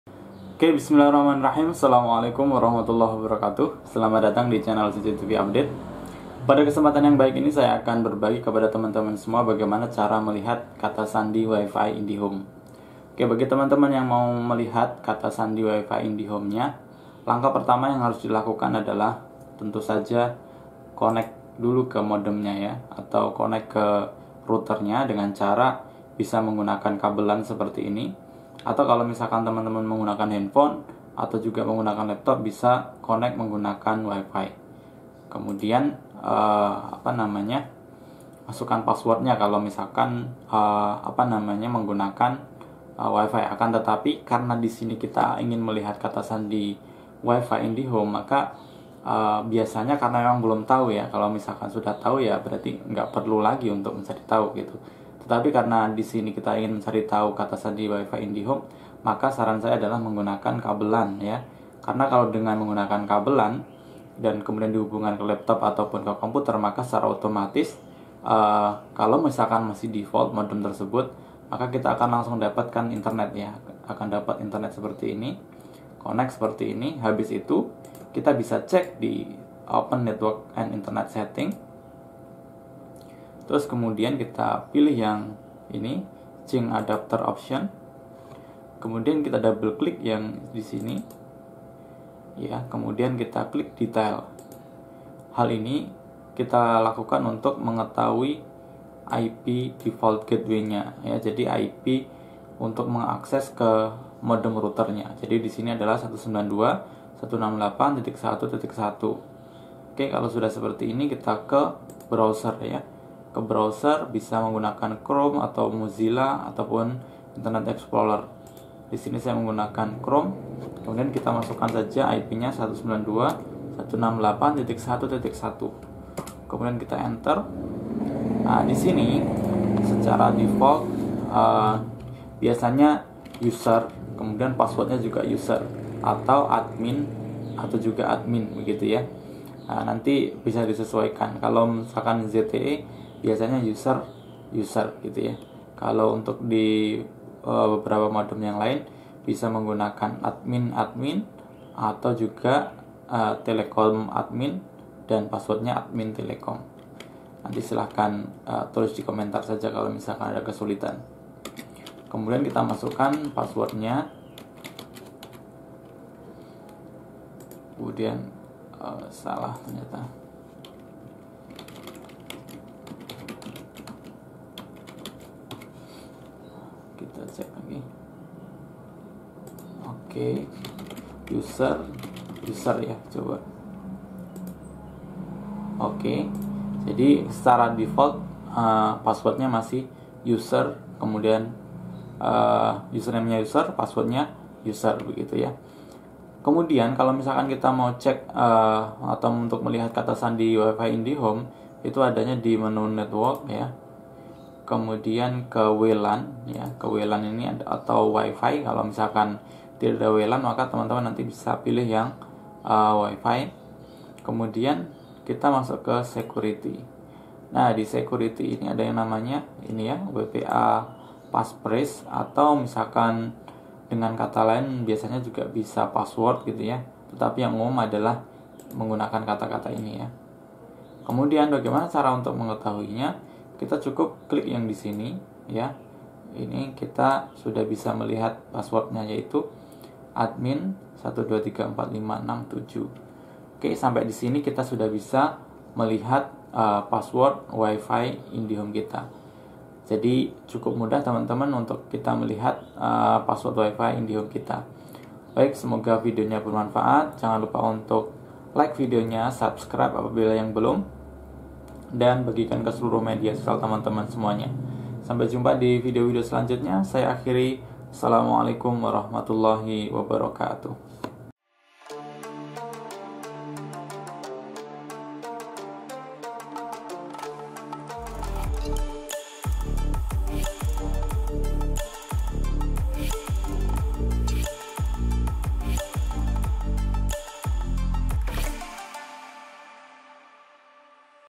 Cara Melihat KATA SANDI WIFI Indihome | Cara Mengetahui PASSWORD WIFI
Rekomendasi CCTV Smart Wifi Ip Camera

Video ini menjelaskan bagaimana Cara Melihat Kata Sandi Wifi Indihome | Cara Mengetahui Password Wifi. Video ini juga terkait dengan cara mengetahui password wifi, cara ganti password wifi indihome, cara melihat kata sandi wifi indihome, dan mengetahui sandi wifi.